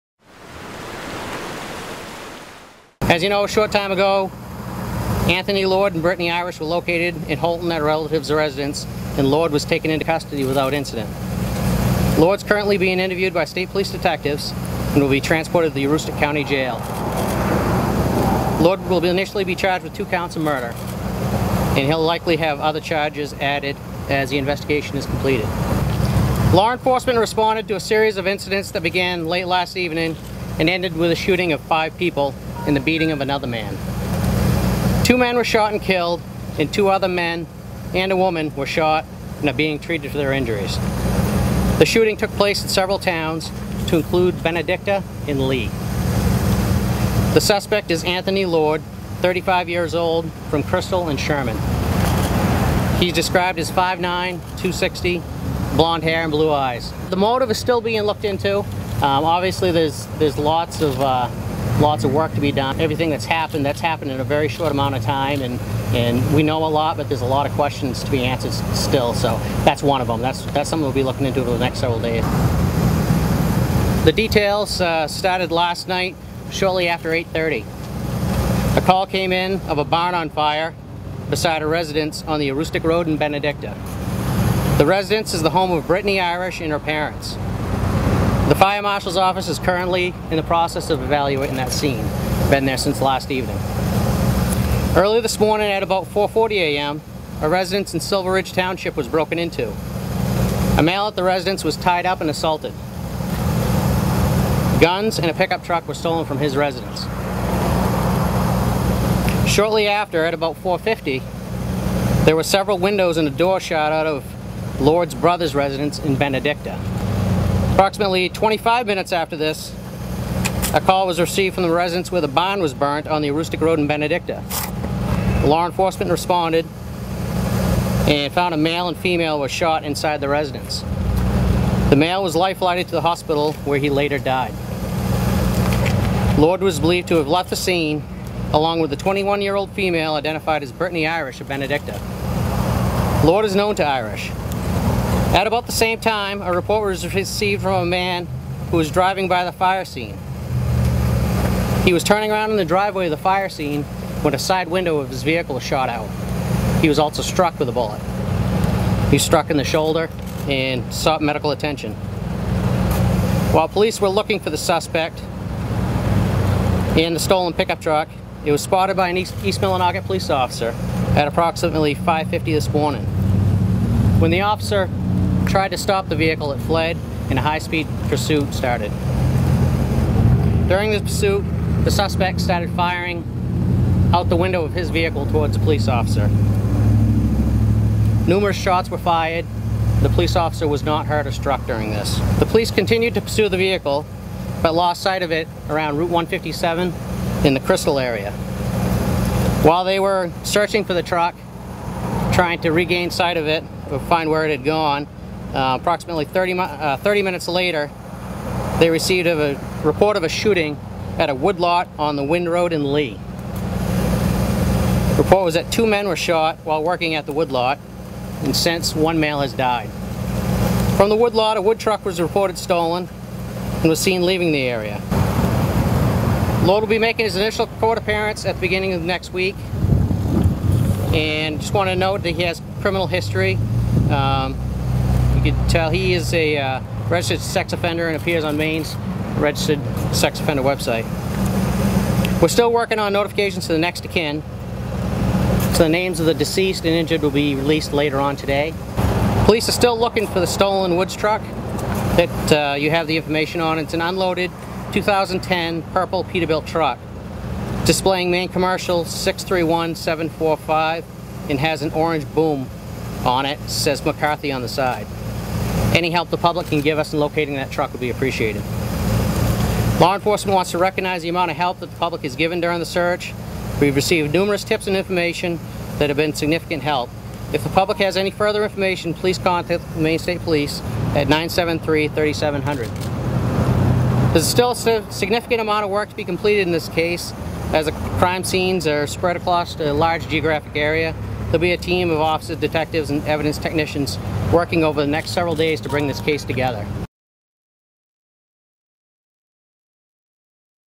Aroostook County man arrested after deadly 18-hour crime spree
Anthony Lord  35  of Crystal and Houlton has been arrested in connection with the shooting of five people in northern Maine. Two people have died. Lord has been charged with two counts of murder. More charges are expected.